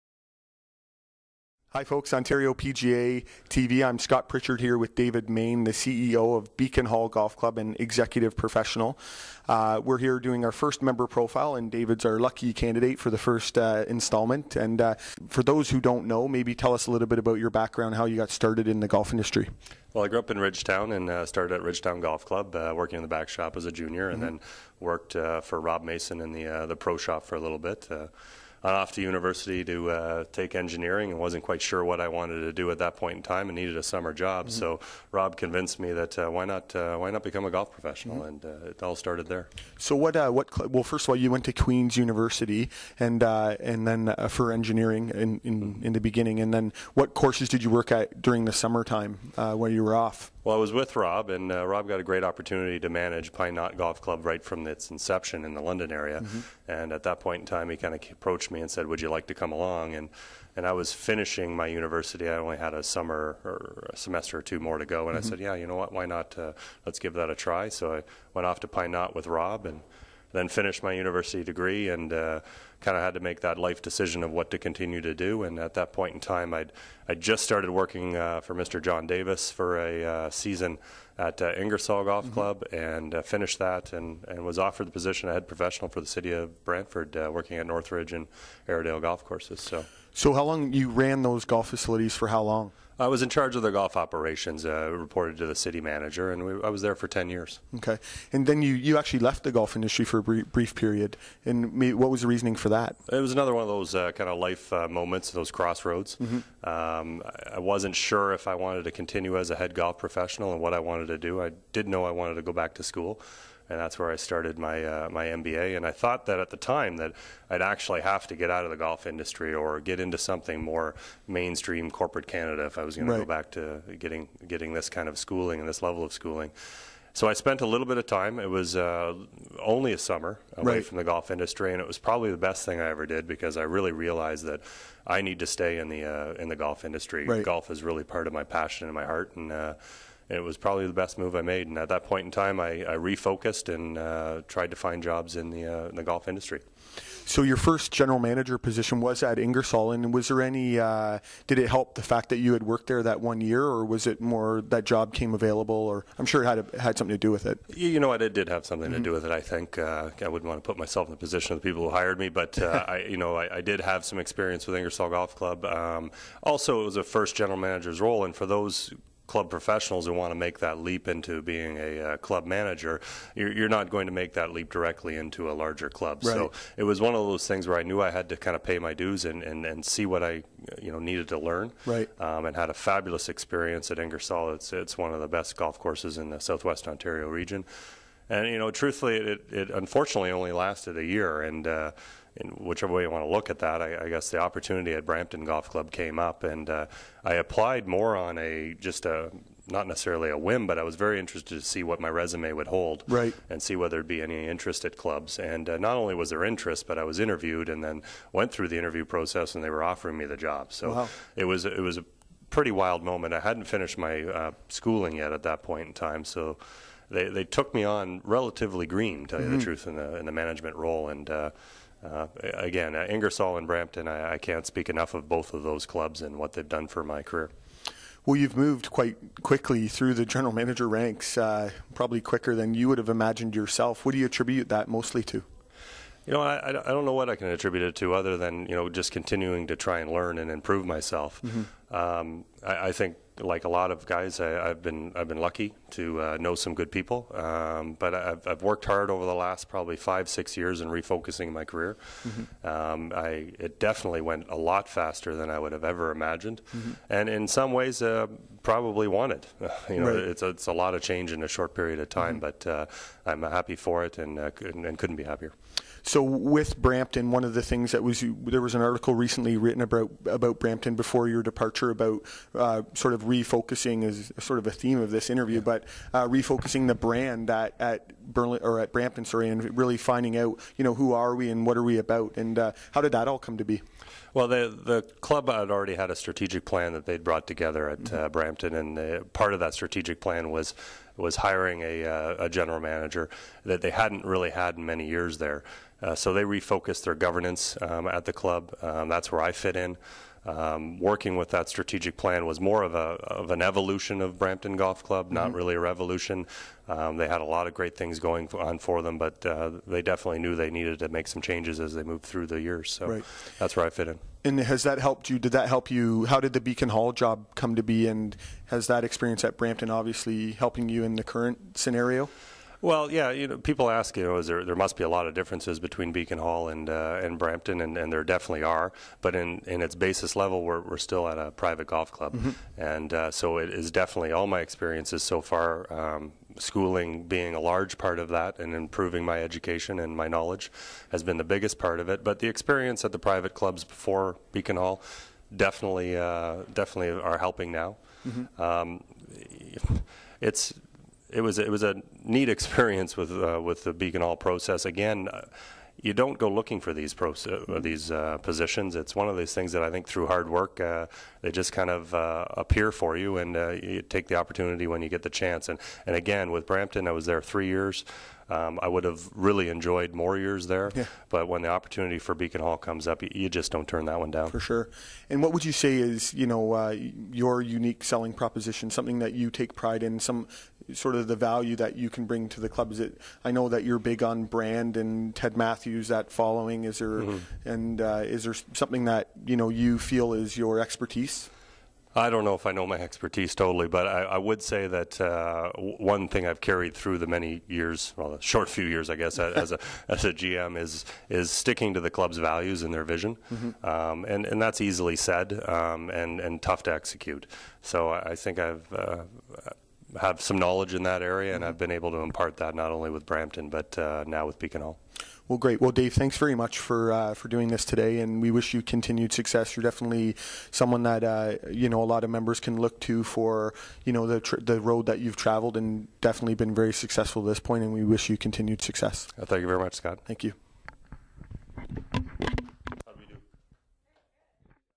Ontario PGA Member Profile - David Main
Ontario PGA TV caught up with David Main, the CEO of Beacon Hall Golf Club to talk about his career and recent move to Beacon Hall